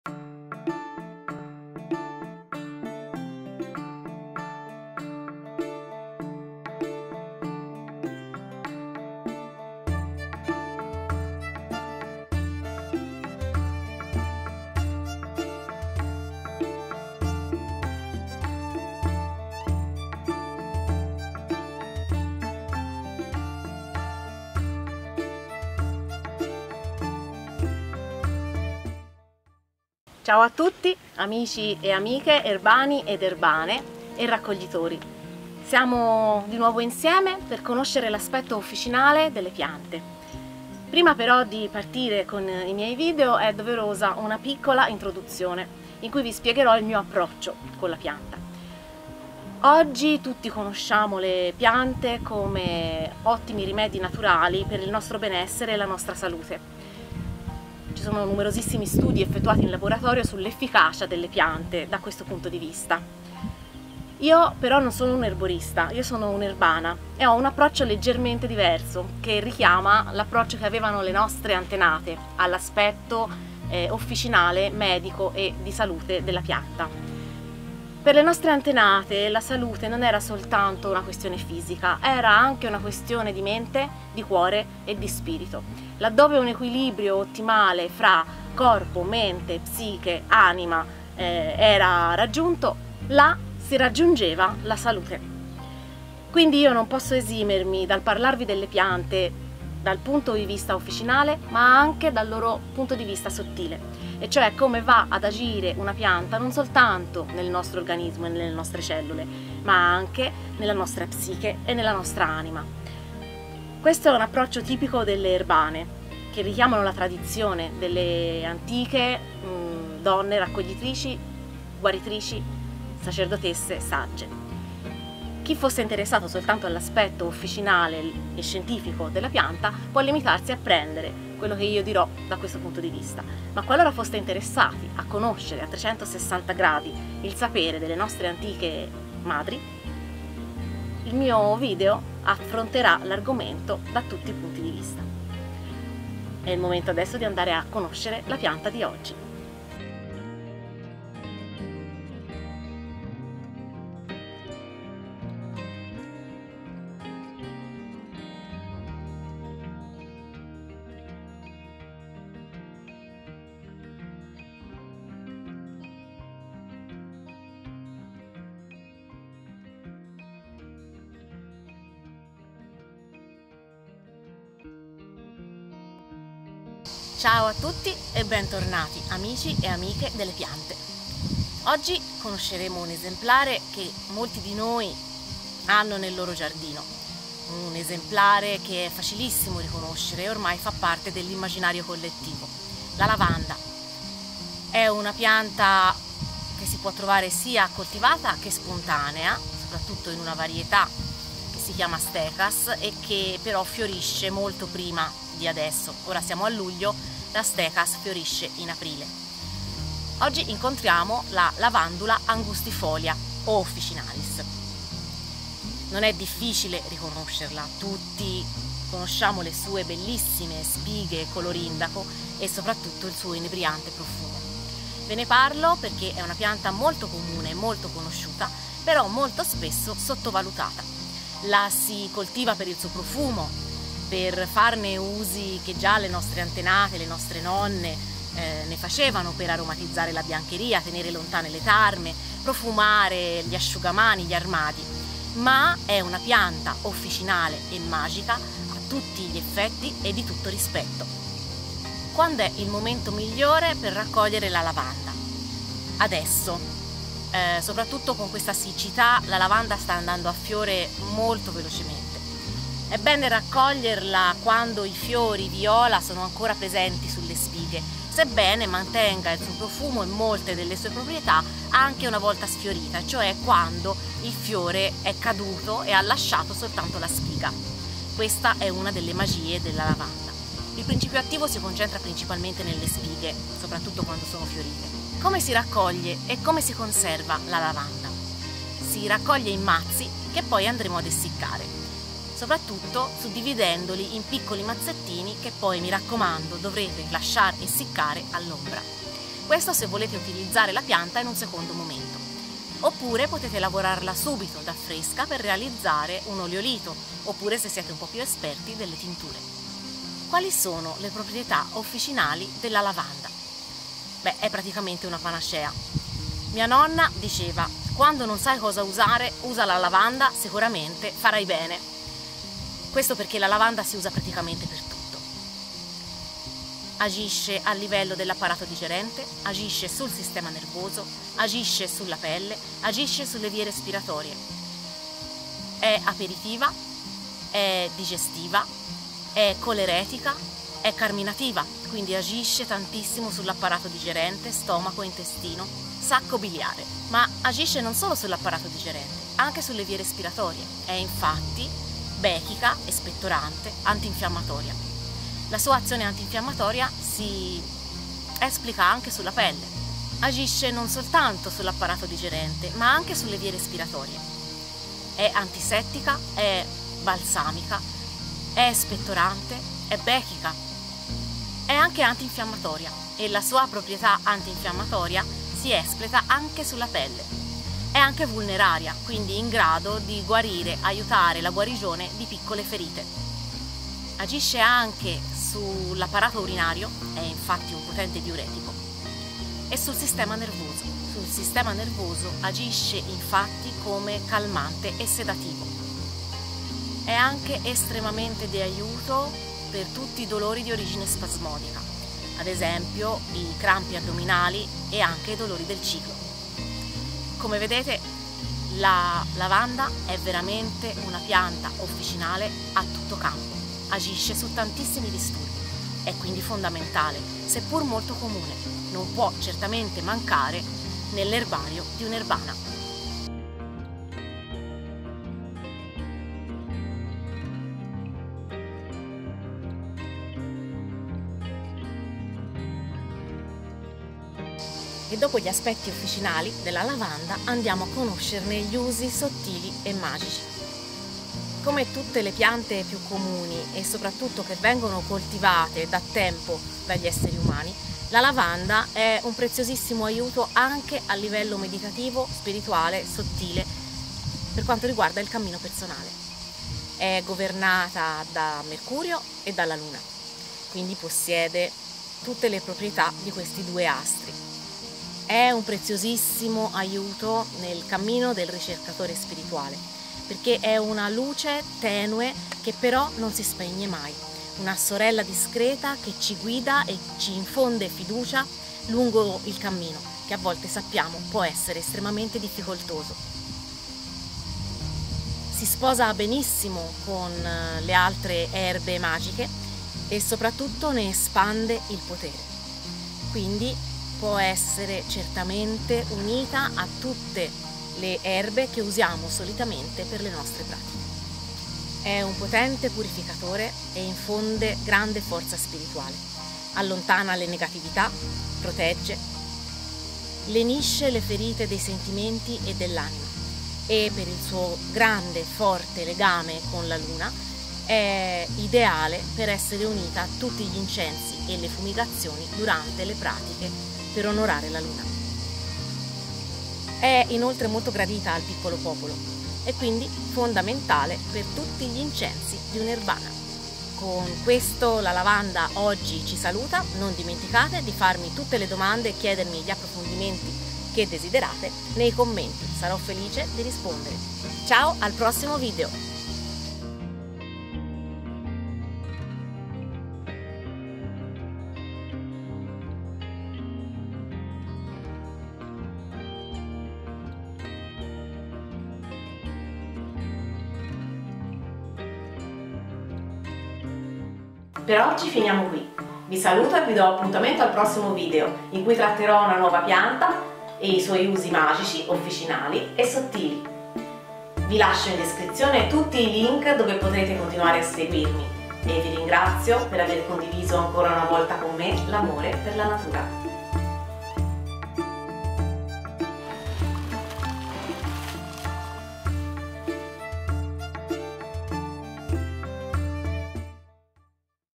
Erbe magiche&officinali: la lavanda
Una pianta che aiuta in tantissime situazioni, una vera panacea, e Fabiola ci guiderà alla sua scoperta!

Video realizzato da Lisa    @SpiritoDellaNatura  ​